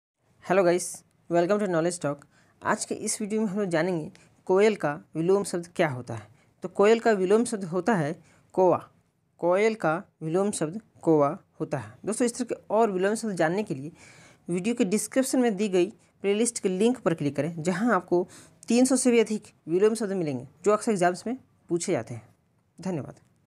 कोयल का विलोम शब्द । Koyal ka vilom shabd । vilom shabd in hindi
Hi

दोस्तों स्वागत है आपका अपने youtube channel  knowledgetalk  में !

दोस्तों आज के इस वीडियो में हमलोग जानेगे की कोयल का विलोम शब्द क्या है !
कोयल के विलोम शब्द -
कौआ